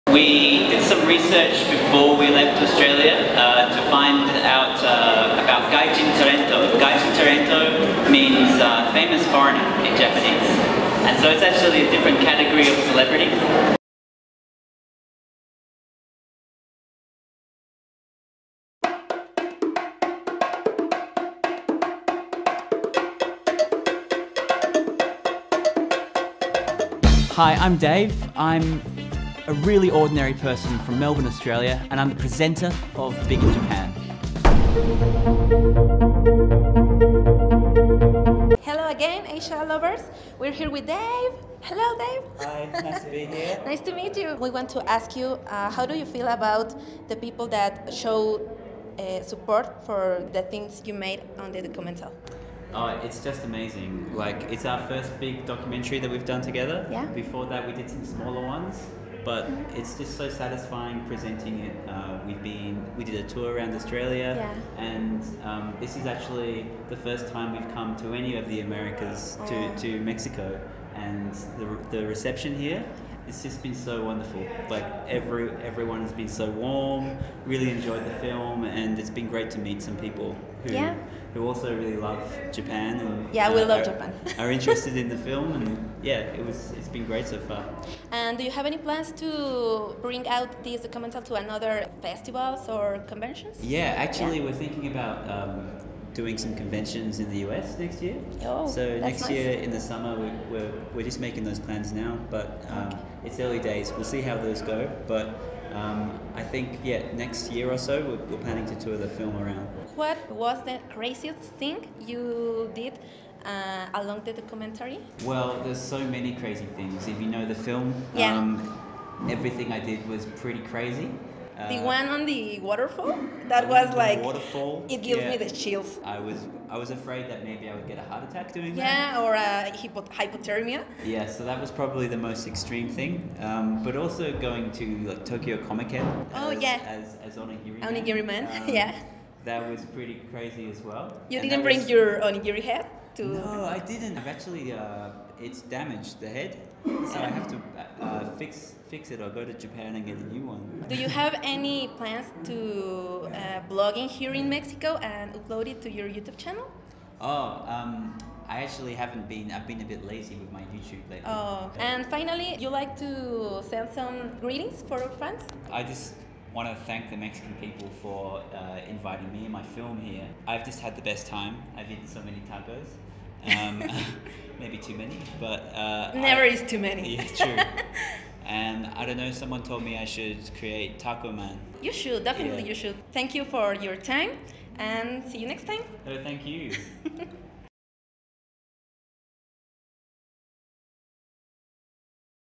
Entrevista con Dave productor de BigInJapan en J'Fest 2019
We had a real 外人タレント infront of us OMG♥

Redes de...